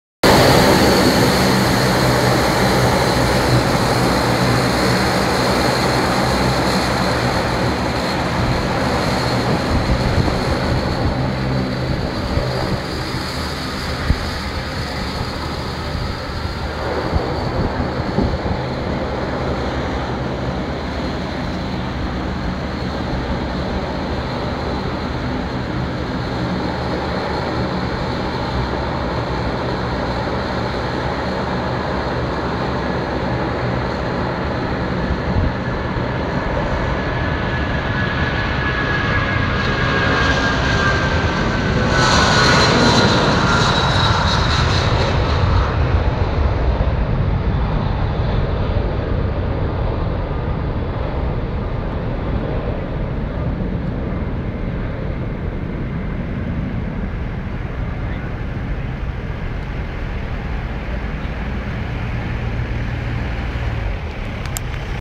Qantas International Flight Take Off
Qantas International Flight at Rockhampton Airport, Queensland, Australia on 12th October, 2009 Captured by Saupriya Solanki.